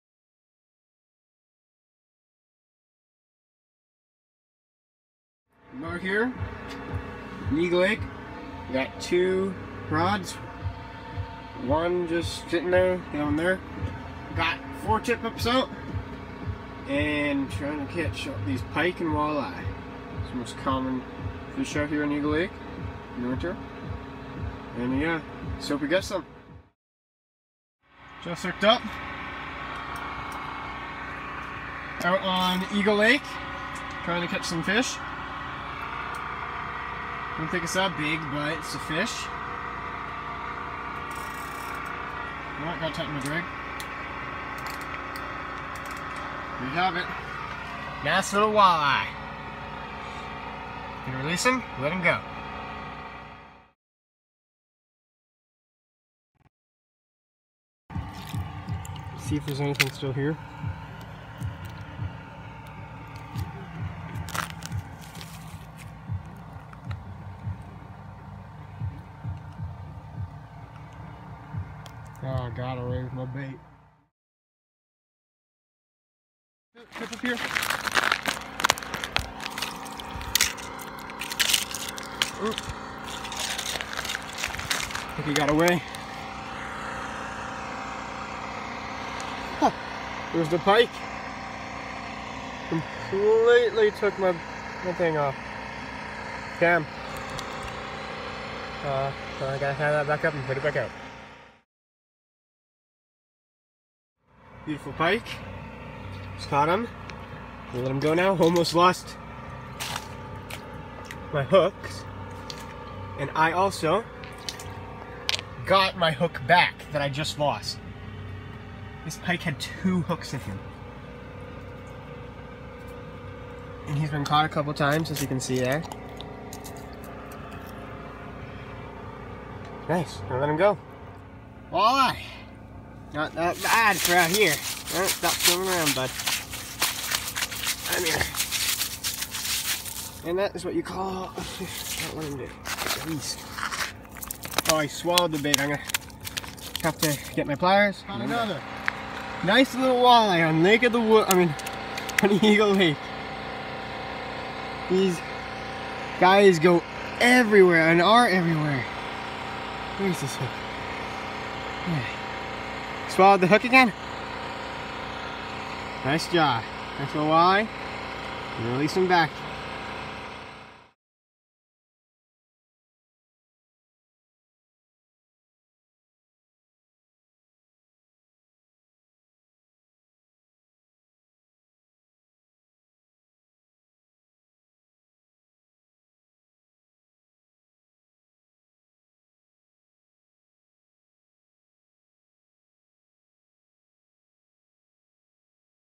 Ice Fishing at eagle lake
I love ice fishing Here the fish are beautiful and nice to fight